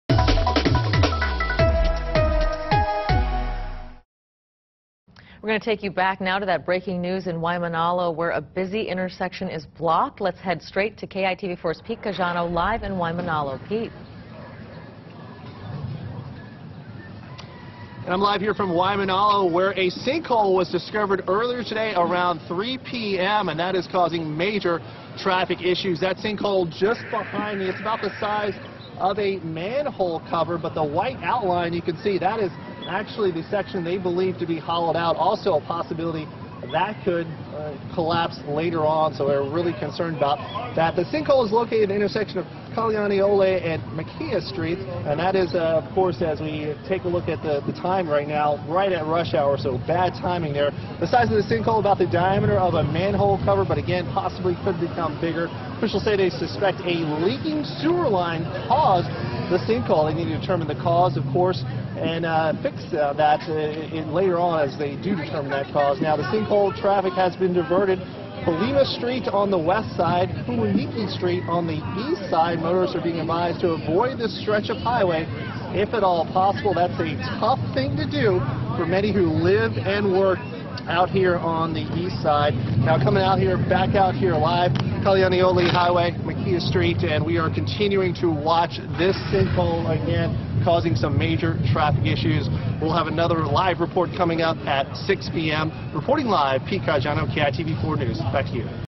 Police close Kalanianaole Hwy. due to sinkhole
Pete Caggiano reports from the scene of the sinkhole in Waimanalo Thursday afternoon.